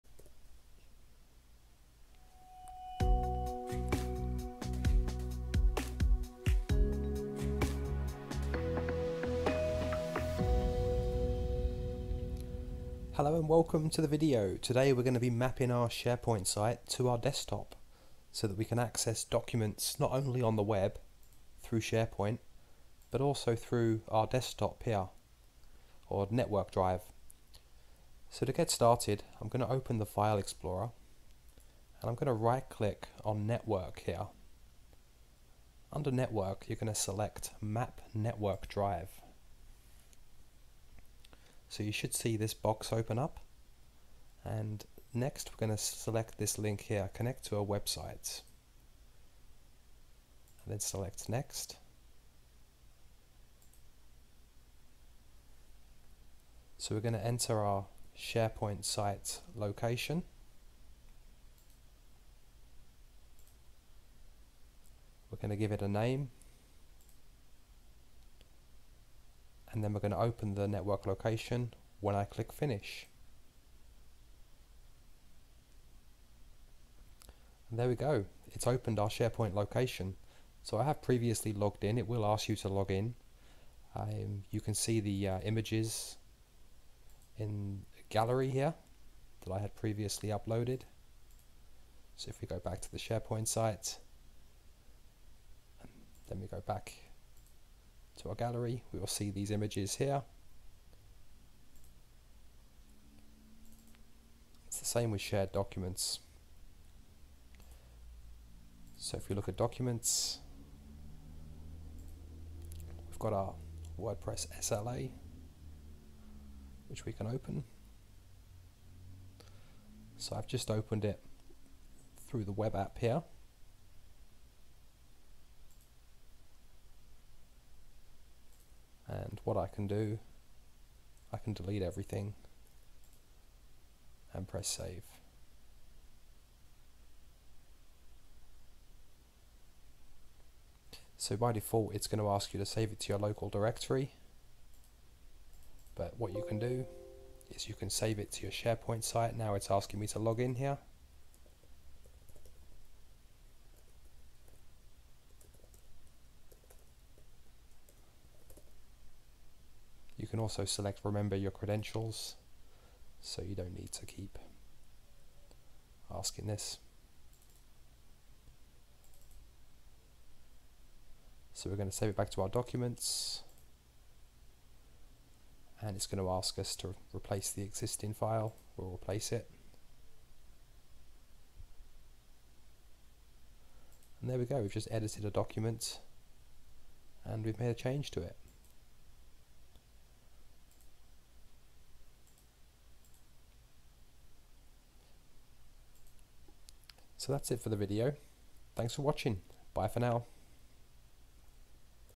How to map SharePoint as a Network Drive on PC
This video will show you how to map SharePoint as a Network Drive on your PC.